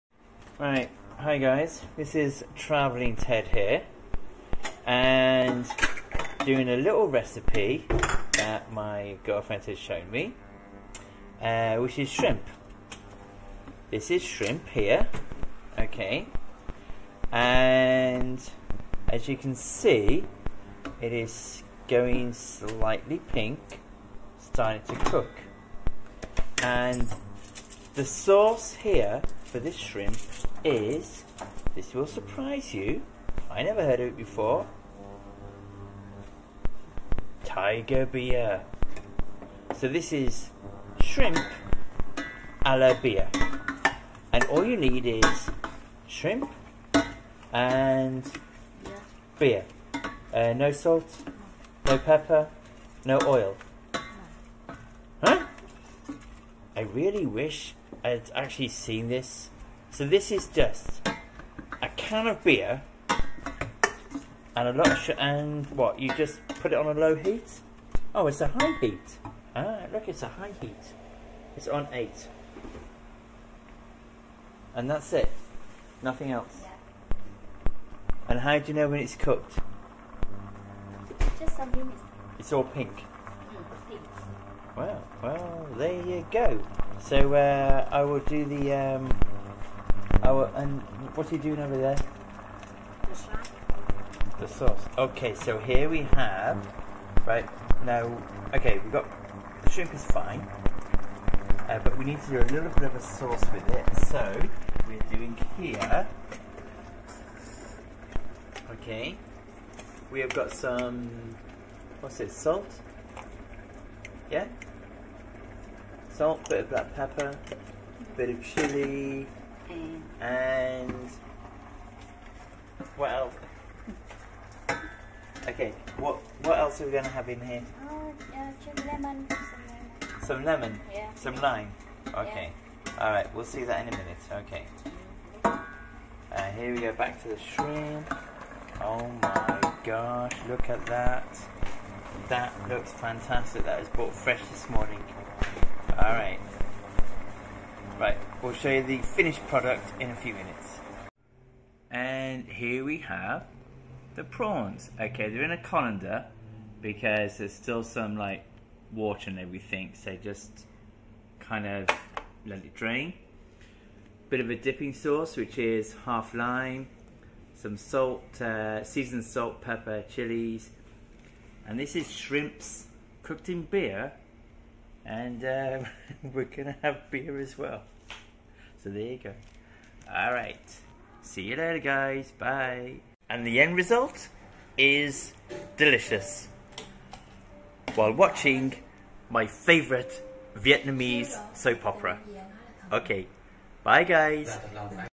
Shrimp cooked in beer - Vietnam style.
Very simple way to cook Shrimp.